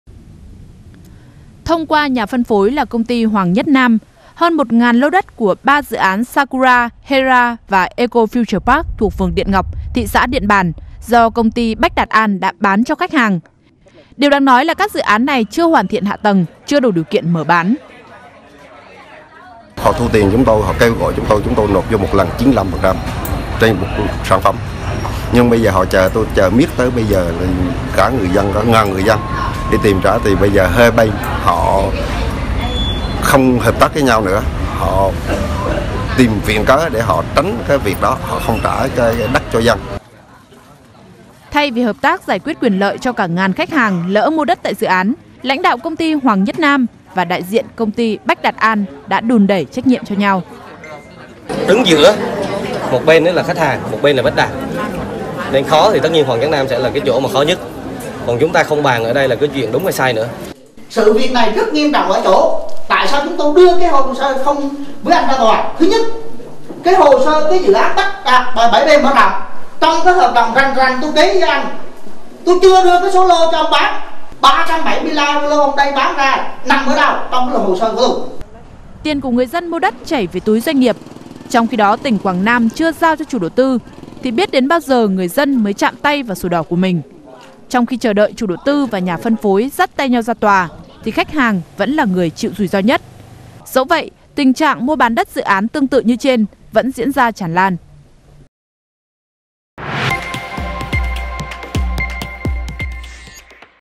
Rủi ro mua đất chưa có sổ đỏ | VTV24
Chưa có sổ đỏ sẽ thiếu cơ sở pháp lý bảo vệ người mua nếu xảy ra tranh chấp